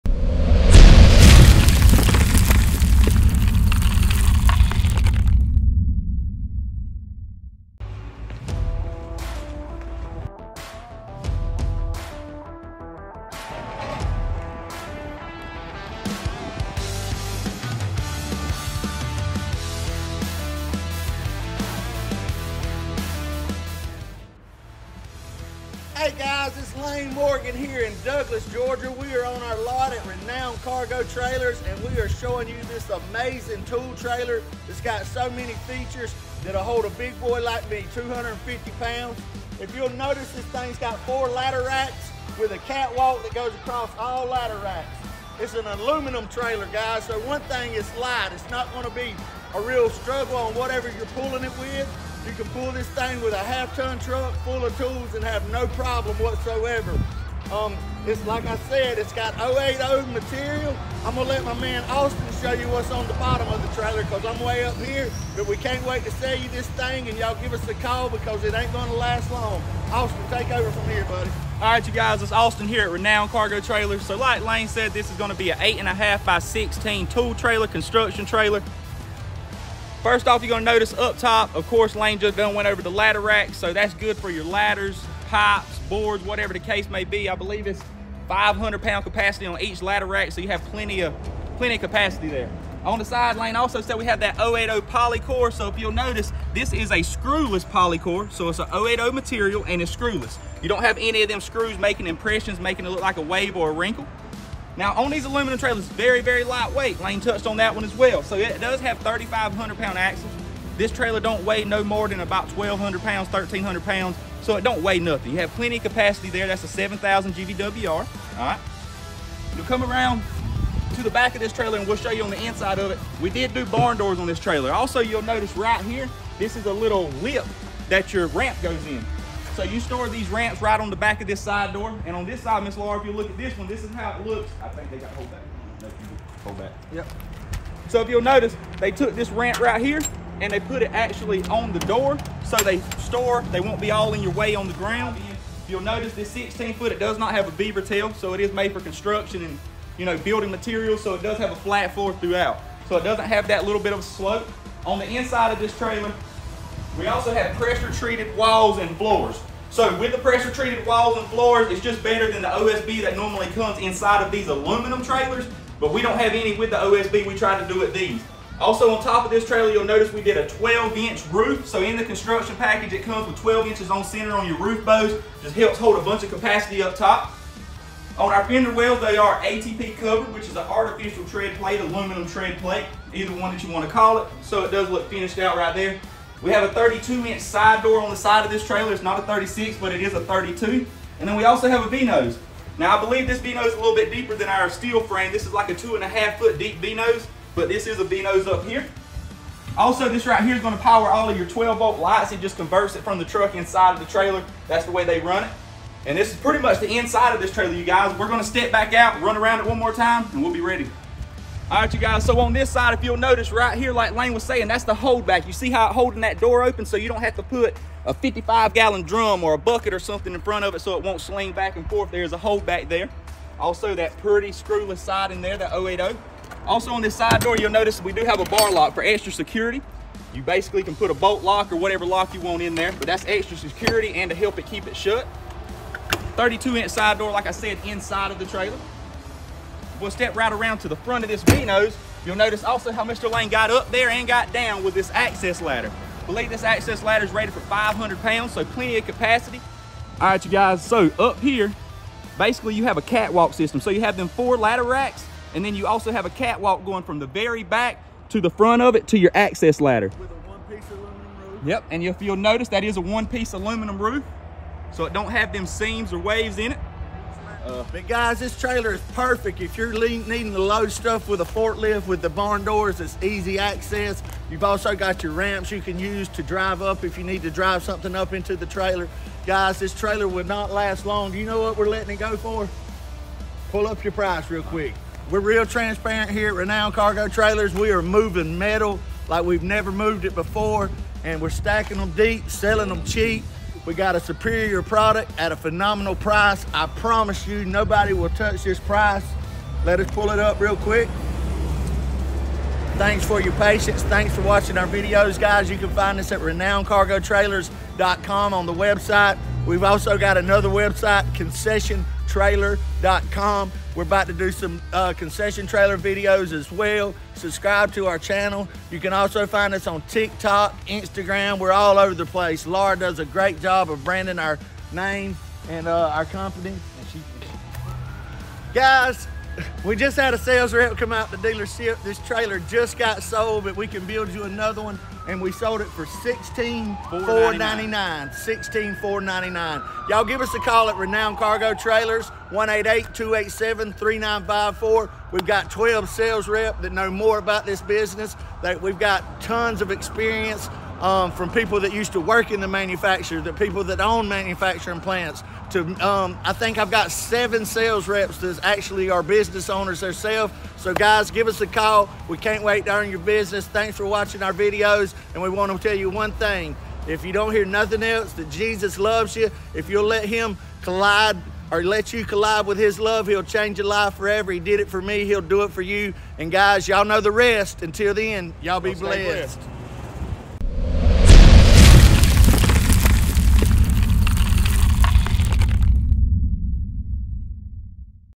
ALCOM EZ-HAULER: Ultimate All-Aluminum Contractor Trailer Walkthrough
Explore the pinnacle of all-aluminum trailers in our latest walkthrough – the ALCOM EZ-HAULER Contractor Trailer. Perfectly crafted for construction and contracting tasks, this aluminum trailer boasts a robust framework, ensuring durability and longevity. With its .080 Polycore exterior, its design is not only seamless but also elegant. The trailer's dual 5200lb axles affirm its strength, making it a standout in the enclosed cargo trailer category. But there's more: the 4-ladder racks, the unmatched security of Blarlock, and the convenient double rear entry. Whether you're in the market for a contractor trailer with ladder racks or just an admirer of top-tier construction trailers, this walkthrough is a must-watch. Dive into the world of ALCOM, EZ-HAULER, and the unmatched quality of aluminum trailers. 👷‍♂️🚚🔧🔩🪜

Your #1 Source for Enclosed Cargo Trailers